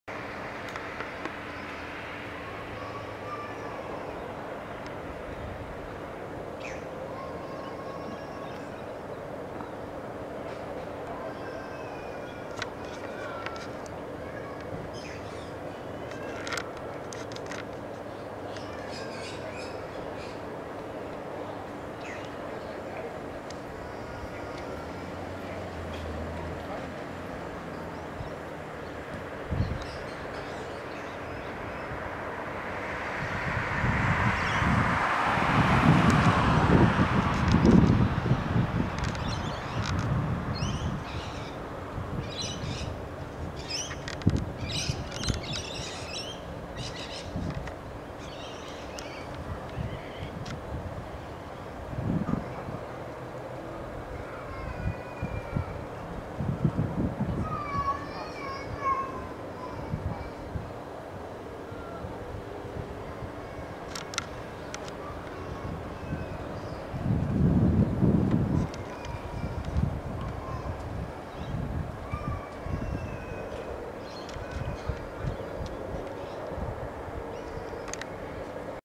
Chemtrails over Brisbane 24/12/2012
The day started off with a beautiful clear blue sky the kind you hardly see anymore but by 3.30 pm there was that much sprayed crap up their i thought it was going to snow.